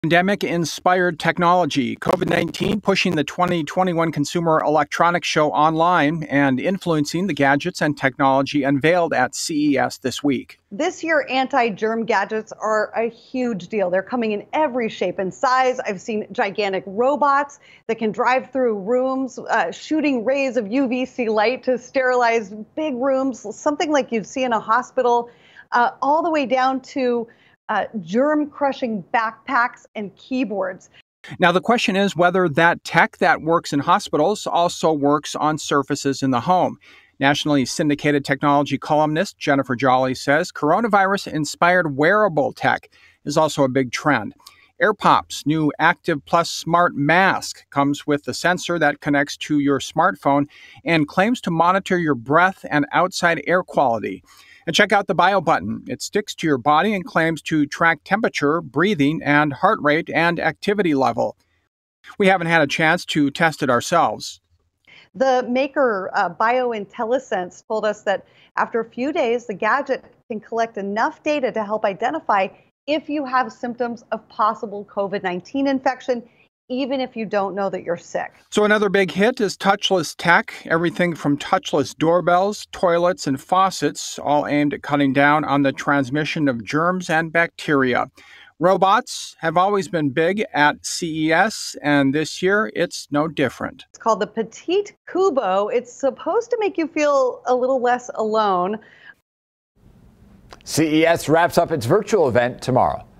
CES unveils smart masks and virus-detecting stickers
CES has unveiled lots of pandemic-related tech from smart masks to virus-detecting stickers.